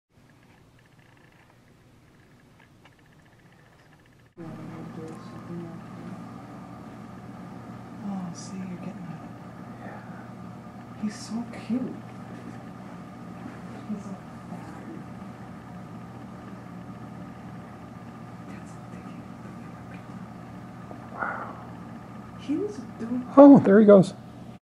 Spotted Owl in the Backyard
Saw a Spotted Owl in the backyard.  This was a first for me.